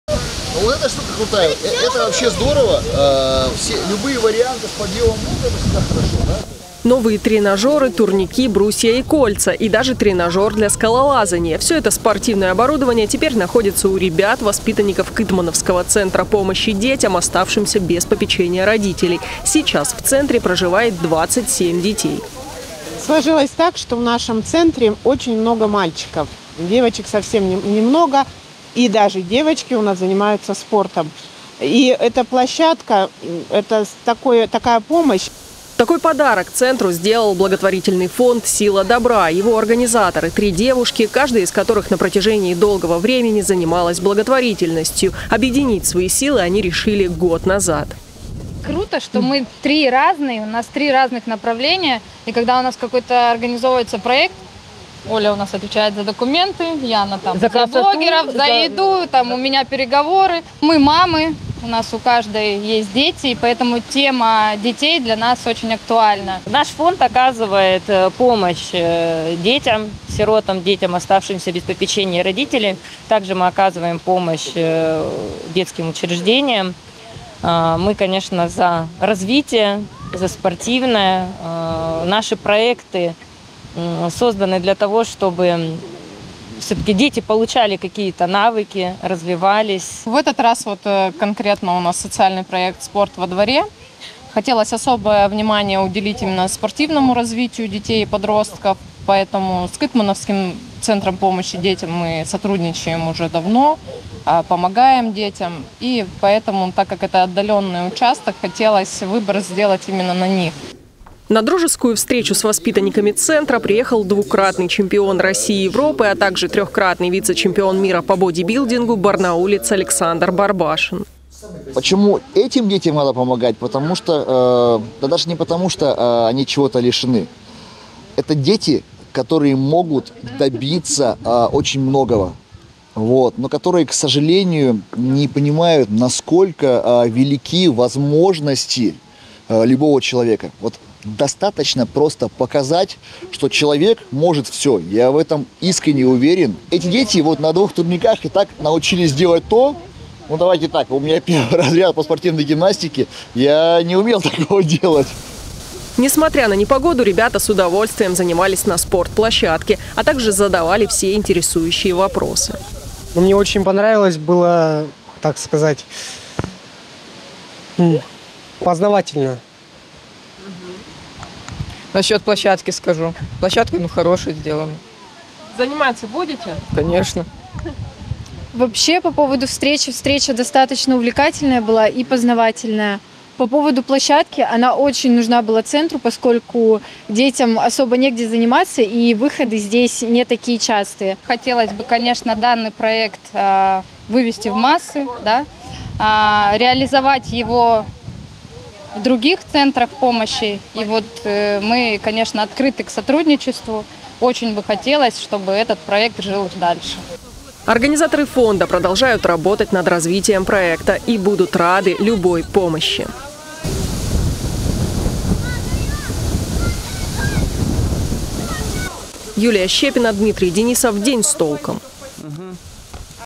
Новая спортивная площадка появилась на территории Кытмановского центра помощи детям
Брусья, кольца, тренажеры – новая спортивная площадка появилась на территории Кытмановского центра помощи детям, оставшимся без попечения родителей. Теперь ребятам, которым приходилось преодолевать расстояние около 3-4 километров до спортивных секций стало проще приобщиться к спорту. Чей это подарок и как к нему отнеслись дети-сироты – смотрите в сюжете.